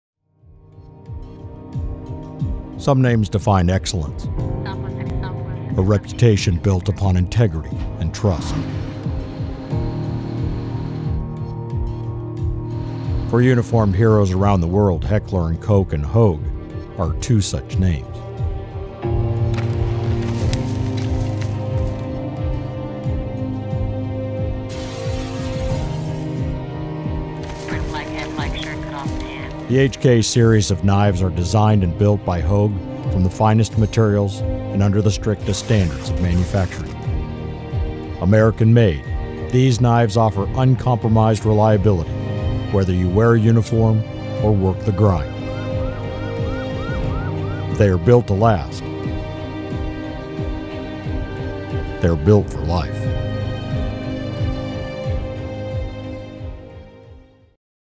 Introducing HK Knives by Hogue Inc.
For over a century combined, Heckler & Koch and Hogue, Inc. have
manufacturing the highest quality firearms and accessories available in the world. These megabrands have joined
forces to release a high quality family of knives.